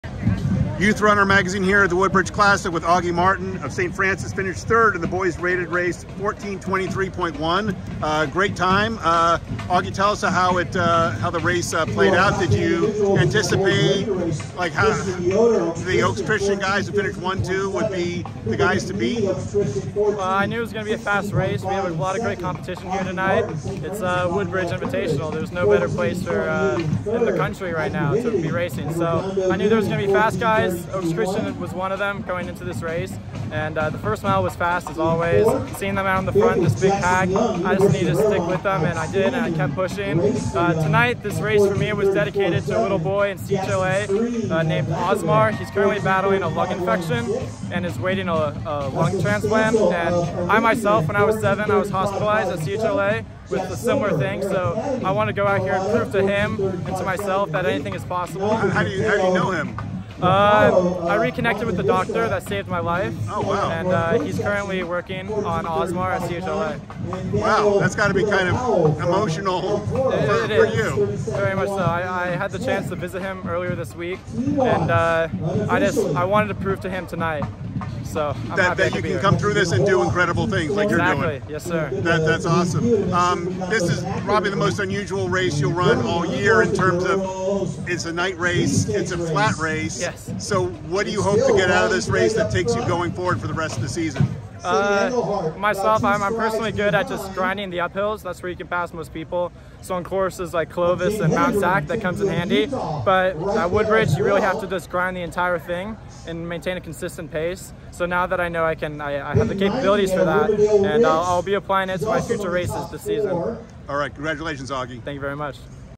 Auge Martin of St. Francis 3rd Place Boys 3-Mile Rated Race at Woodbridge Classic
Auge Martin, a senior at St. Francis High in California, took third in the boys varsity 3-mile rated race in 14:23.1 at the 43rd Woodbridge Cross Country Classic Presented by ASICS America at Great Park in Irvine, Calif. Martin helped the Golden Knights secure 16th place with 452 points.